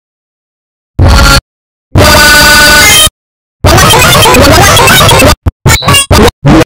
my pitch method
heres how i do pitches for my remixes